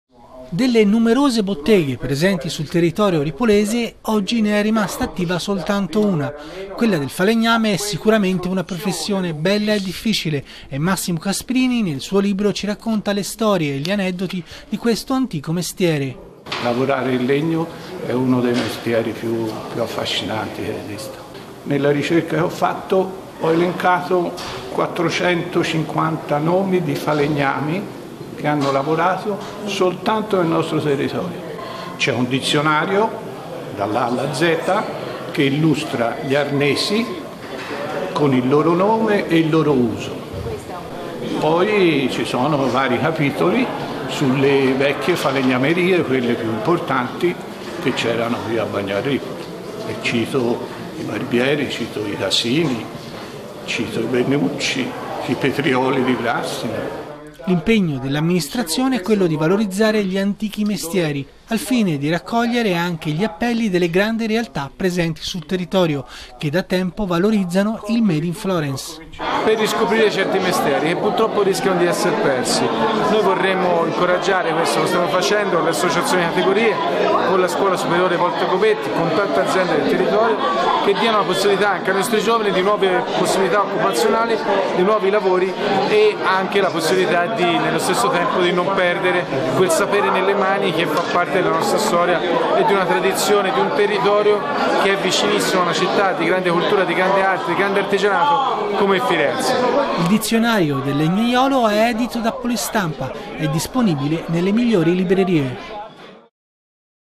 Il dizionario del legnaiolo, viaggio alla ricerca di un antico mestiere
Delle numerose botteghe presenti sul territorio ripolese oggi ne è rimasta attiva soltato una, quella del falegname è una professione  bella  e difficile, e Massimo Casprini nel suo libro ci racconta le storie e gli aneddoti  di questo antico mestiere.
“Nella ricerca che ho fatto ho elencato  450 nomi di falegnami” – racconta Massimo Casprini – “che hanno lavorato nel nostro territorio. Inoltre è presente un dizionario dalla A alla Z che illustra gli arnesi con il loro nome ed il loro uso. Vari capitoli sono dedicati alle vecchie falegnamerie più imortanti tra le quali i Barbieri, i Casini, i Benucci, i Petrioli”
L’impegno dell’amministrazione è quello di valorizzare gli antichi mestieri al fine di raccogliere anche gli appelli delle grandi realtà presenti sul territorio, che da tempo valorizzano il made il florence.
“Riscoprire i vecchi mestieri è un impegno della nostra amministrazione”- racconta Francesco Casini – “noi stiamo portando avanti alcuni progetti con le associazioni di categoria, con la scuola superiore Volta Gobetti e con molte aziende del territorio al fine di dare ai nostri giovani nuove possibilità occupazionali. Nello stesso tempo anche la possibilità dei non perdere quel “sapere nelle mani” che fa parte della nostra storia e di una tradizione di un territorio vicino ad una grande città di grande cultura ed ingegno come Firenze.”
Il dizionario del legnaiolo è edito da Polistampa e disponbile nelle migliori librerie.